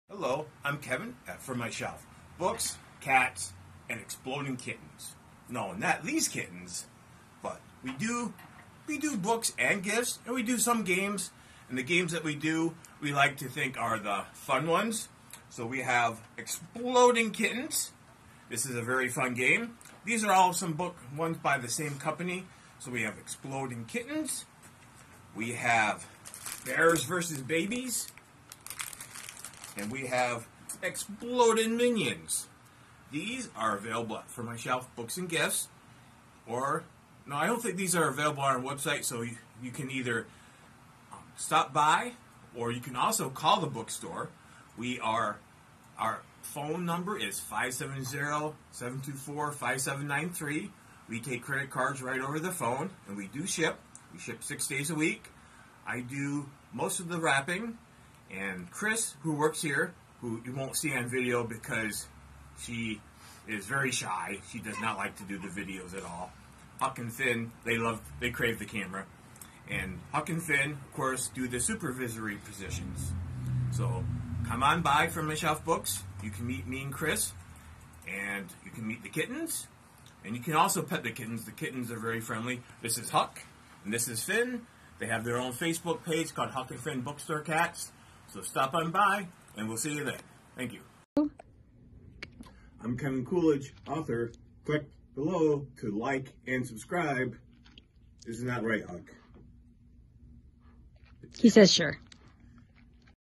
Bears, Babies, & Exploding Minions!
Kevin and the boys talk about Exploding Minions and Bears vs Babies!!!